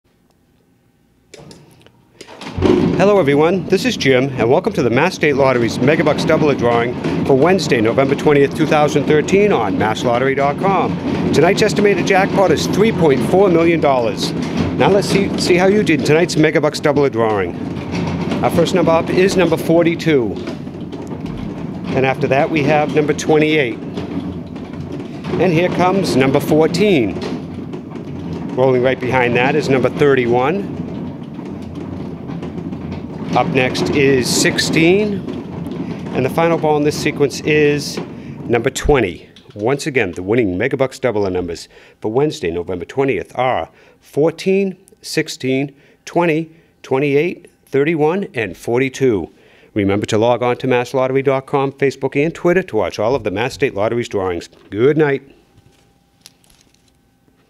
Megabucks Doubler Game Drawing: Wednesday, November 20, 2013
The official winning numbers in the Mass. State Lottery's Megabucks Doubler Game drawing for Wednesday, November 20, 2013 are: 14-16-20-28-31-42.